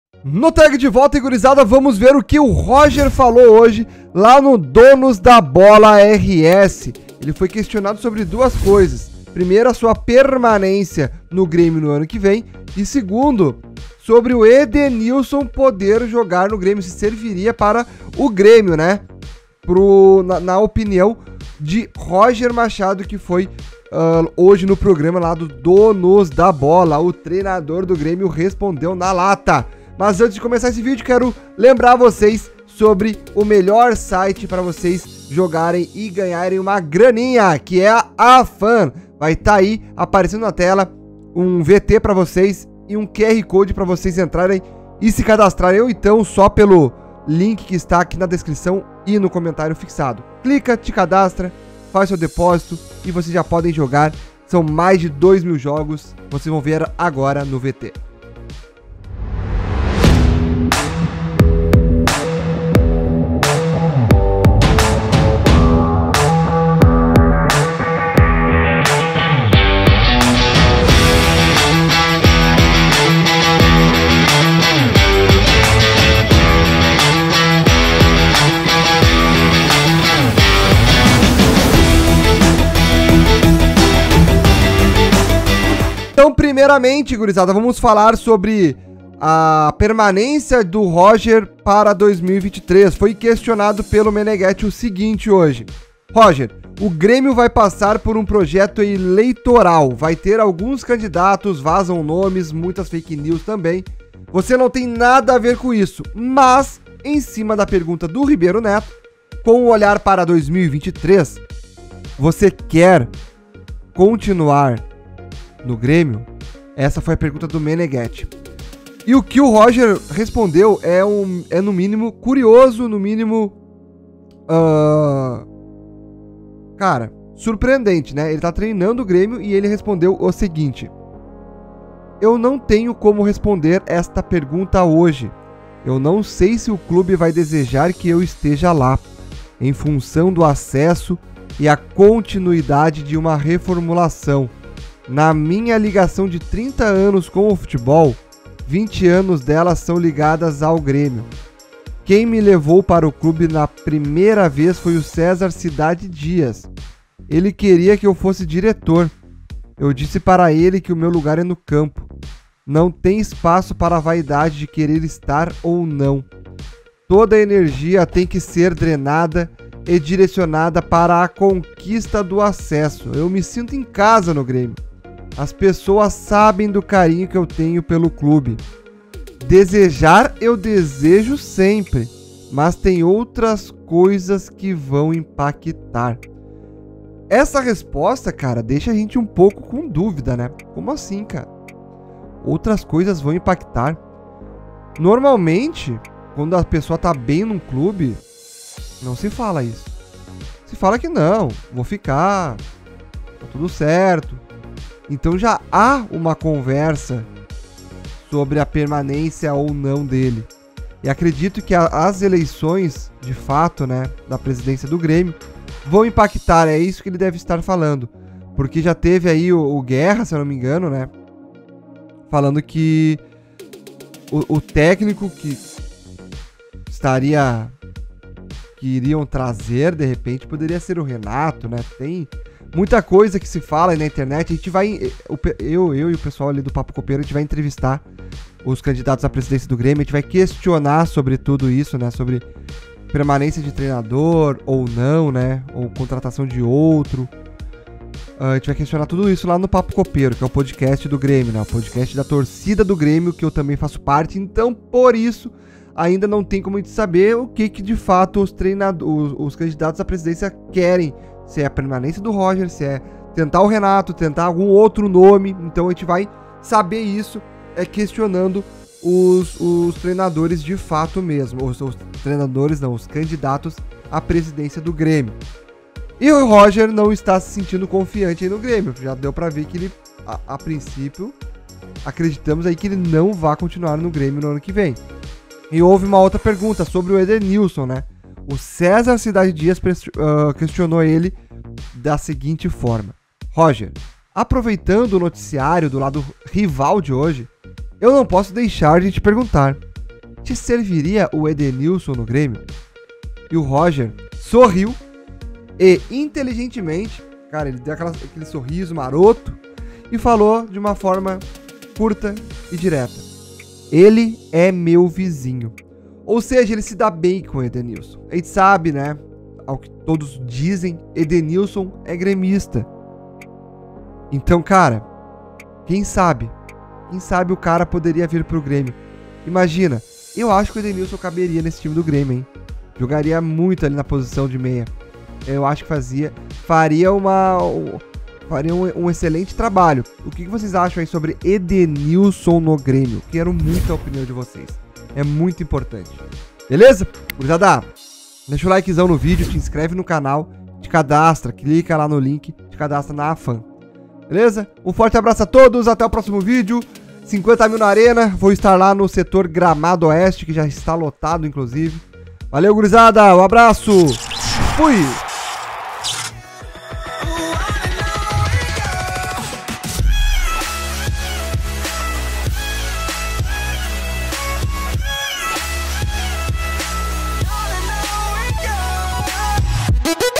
ROGER FALA SE VAI FICAR NO GRÊMIO ANO QUE VEM E SOBRE EDENÍLSON NO GRÊMIO!
Roger falou algumas coisas importantes hoje sbore o Grêmio!